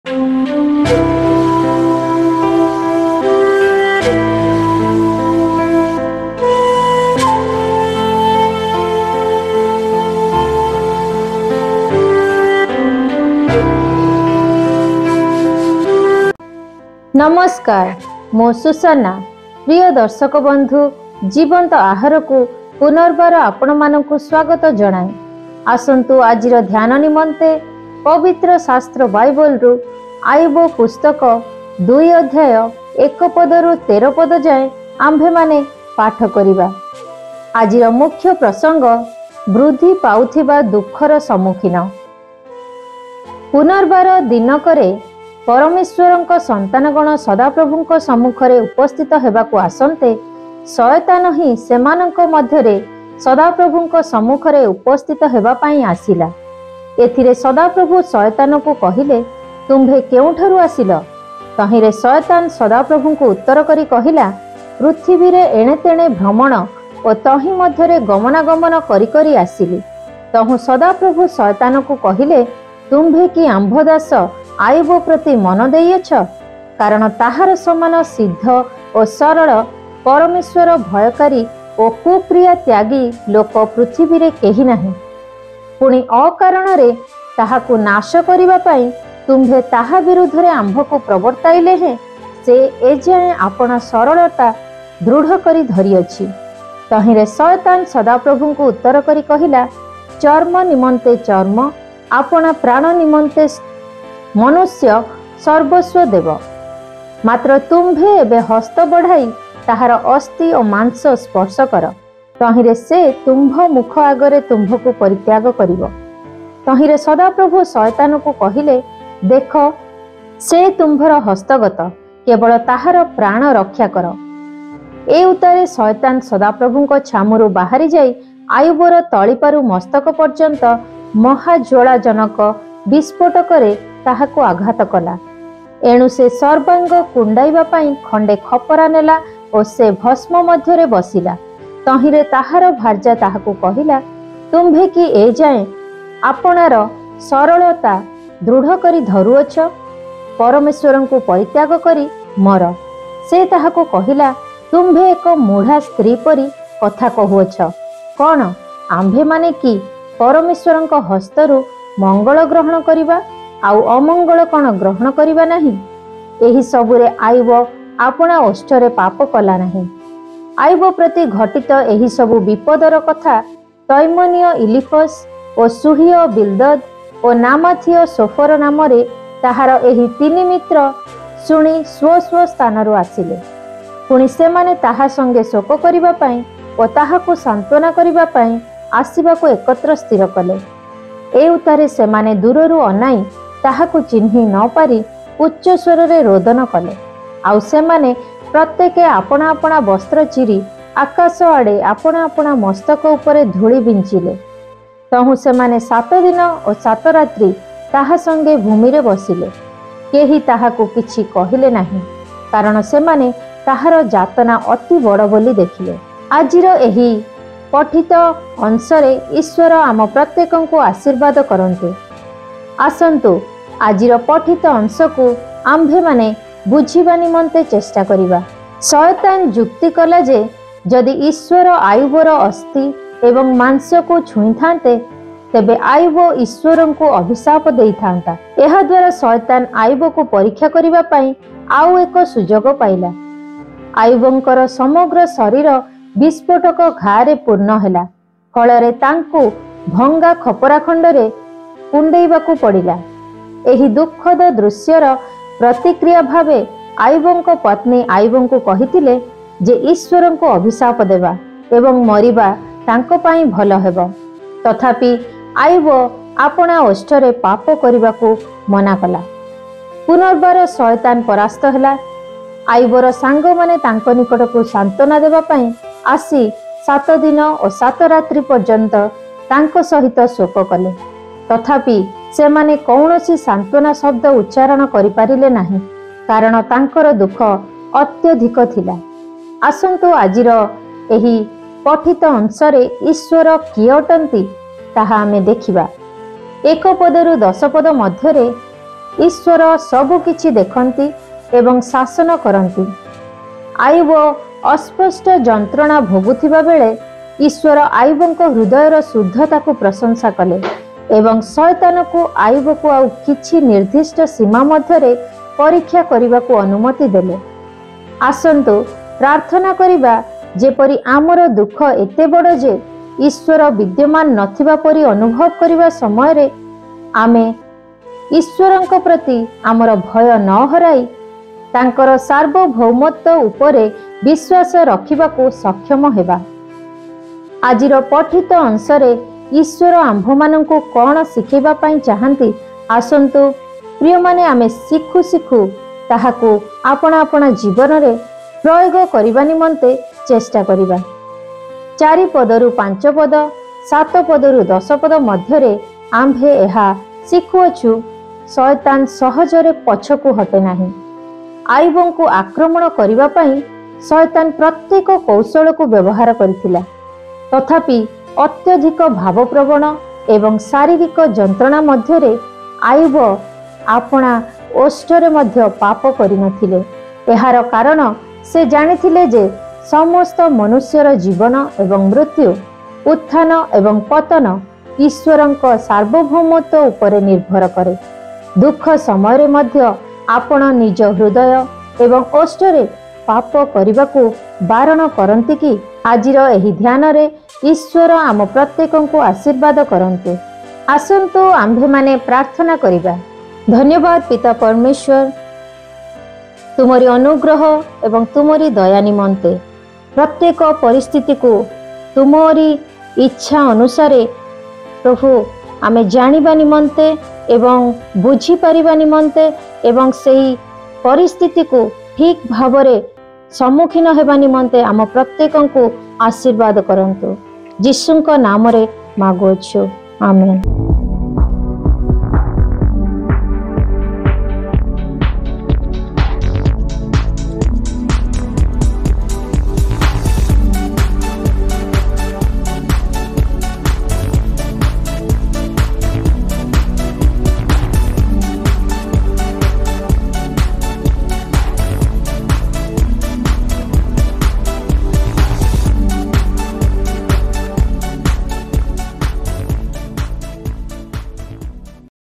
THE BREAD OF LIFE 05 NOV 2023
ଆୟୁବ 2:1-13
ଆୟୁବଙ୍କ ସ୍ଵାସ୍ଥ୍ୟରେ ଶୟତାନର ଆକ୍ରମଣ
1ପୁନର୍ବାର ଦିନକରେ ପରମେଶ୍ଵରଙ୍କ ସନ୍ତାନଗଣ ସଦାପ୍ରଭୁଙ୍କ ସମ୍ମୁଖରେ ଉପସ୍ଥିତ ହେବାକୁ ଆସନ୍ତେ, ଶୟତାନ ହିଁ ସେମାନଙ୍କ ମଧ୍ୟରେ ସଦାପ୍ରଭୁଙ୍କ ସମ୍ମୁଖରେ ଉପସ୍ଥିତ ହେବା ପାଇଁ ଆସିଲା। 2ଏଥିରେ ସଦାପ୍ରଭୁ ଶୟତାନକୁ କହିଲେ, ତୁମ୍ଭେ କେଉଁଠାରୁ ଆସିଲ? ତହିଁରେ ଶୟତାନ ସଦାପ୍ରଭୁଙ୍କୁ ଉତ୍ତର କରି କହିଲା, ପୃଥିବୀରେ  ଣେତେଣେ ଭ୍ରମଣ ଓ ତହିଁ ମଧ୍ୟରେ ଗମନାଗମନ କରି କରି ଆସିଲି। 3ତହୁଁ ସଦାପ୍ରଭୁ ଶୟତାନଙ୍କୁ କହିଲେ, ତୁମ୍ଭେ କି ଆମ୍ଭ ଦାସ ଆୟୁବ ପ୍ରତି ମନ ଦେଇଅଛ? କାରଣ ତାହାର ସମାନ ସିଦ୍ଧ ଓ ସରଳ, ପରମେଶ୍ଵର-ଭୟକାରୀ ଓ  କ୍ରିୟାତ୍ୟାଗୀ ଲୋକ ପୃଥିବୀରେ କେହି ନାହିଁ; ପୁଣି, ଅକାରଣରେ ତାହାକୁ ନାଶ କରିବା ପାଇଁ ତୁମ୍ଭେ ତାହା ବିରୁଦ୍ଧରେ ଆମ୍ଭକୁ ପ୍ରବର୍ତ୍ତାଇଲେ ହେଁ ସେ ଏଯାଏ ଆପଣା ସରଳତା ଦୃଢ଼ କରି ଧରିଅଛି। 4ତହିଁରେ ଶୟତାନ ସଦାପ୍ରଭୁଙ୍କୁ ଉତ୍ତର କରି କହିଲା, ଚର୍ମ ନିମନ୍ତେ ଚର୍ମ, ଆପଣା ପ୍ରାଣ ନିମନ୍ତେ ମନୁଷ୍ୟ ସର୍ବସ୍ଵ ଦେବ। 5ମାତ୍ର ତୁମ୍ଭେ ଏବେ ହସ୍ତ ବଢ଼ାଇ ତାହାର ଅସ୍ଥି ଓ ମାଂସ ସ୍ପର୍ଶ କର, ତହିଁରେ ସେ ତୁମ୍ଭ ମୁଖ ଆଗରେ ତୁମ୍ଭକୁ ପରିତ୍ୟାଗ କରିବ। 6ତହିଁରେ ସଦାପ୍ରଭୁ ଶୟତାନଙ୍କୁ କହିଲେ, ଦେଖ, ସେ ତୁମ୍ଭର ହସ୍ତଗତ, କେବଳ ତାହାର ପ୍ରାଣ ରକ୍ଷା କର। 7ଏଉତ୍ତାରେ ଶୟତାନ ସଦାପ୍ରଭୁଙ୍କ ଛାମୁରୁ ବାହାରି ଯାଇ ଆୟୁବର ତଳିପାରୁ ମସ୍ତକ ପର୍ଯ୍ୟନ୍ତ ମହାଜ୍ଵାଳାଜନକ ବିସ୍ଫୋଟକରେ ତାହାକୁ ଆଘାତ କଲା। 8ଏଣୁ ସେ ସର୍ବାଙ୍ଗ କୁଣ୍ତାଇବା ପାଇଁ ଖଣ୍ତେ ଖପରା ନେଲା ଓ ସେ ଭସ୍ମ ମଧ୍ୟରେ ବସିଲା। 9ତହିଁରେ ତାହାର ଭାର୍ଯ୍ୟା ତାହାକୁ କହିଲା, ତୁମ୍ଭେ କି ଏଯାଏ ଆପଣାର ସରଳତା ଦୃଢ଼ କରି ଧରୁଅଛ? ପରମେଶ୍ଵରଙ୍କୁ ପରିତ୍ୟାଗ କରି ମର। 10ସେ ତାହାକୁ କହିଲା, ତୁମ୍ଭେ ଏକ ମୂଢ଼ା ସ୍ତ୍ରୀ ପରି କଥା କହୁଅଛଣ! କଅଣ? ଆମ୍ଭେମାନେ କି ପରମେଶ୍ଵରଙ୍କ ହସ୍ତରୁ ମଙ୍ଗଳ ଗ୍ରହଣ କରିବା? ଆଉ, ଅମଙ୍ଗଳ କʼଣ ଗ୍ରହଣ କରିବା ନାହିଁ? ଏହିସବୁରେ ଆୟୁବ ଆପଣା ଓଷ୍ଠରେ ପାପ କଲା ନାହିଁ।  11ଆୟୁବ ପ୍ରତି ଘଟିତ ଏହିସବୁ ବିପଦର କଥା ତୈମନୀୟ ଇଲୀଫସ୍ ଓ ଶୂହୀୟ ବିଲ୍ଦଦ୍ ଓ ନାମାଥୀୟ ସୋଫର ନାମରେ ତାହାର ଏହି ତିନି ମିତ୍ର ଶୁଣି ସ୍ଵ ସ୍ଵ ସ୍ଥାନରୁ ଆସିଲେ; ପୁଣି, ସେମାନେ ତାହା ସଙ୍ଗେ ଶୋକ କରିବା ପାଇଁ ଓ ତାହାକୁ ସାନ୍ତ୍ଵନା କରିବା ପାଇଁ ଆସିବାକୁ ଏକତ୍ର ସ୍ଥିର କଲେ। 12ଏଉତ୍ତାରେ ସେମାନେ ଦୂରରୁ ଅନାଇ ତାହାକୁ ଚିହ୍ନି ନ ପାରି ଉଚ୍ଚୈଃସ୍ଵରରେ ରୋଦନ କଲେ; ଆଉ, ସେମାନେ ପ୍ରତ୍ୟେକେ ଆପଣା ଆପଣା ବସ୍ତ୍ର ଚିରି ଆକାଶ ଆଡ଼େ ଆପଣା ଆପଣା ମସ୍ତକ ଉପରେ ଧୂଳି ବିଞ୍ଚିଲେ। 13ତହୁଁ ସେମାନେ ସାତ ଦିନ ଓ ସାତ ରାତ୍ରି ତାହା ସଙ୍ଗେ ଭୂମିରେ ବସିଲେ, କେହି ତାହାକୁ କିଛି କହିଲେ ନାହିଁ; କାରଣ ସେମାନେ ତାହାର ଯାତନା ଅତି ବଡ଼ ବୋଲି ଦେଖିଲେ।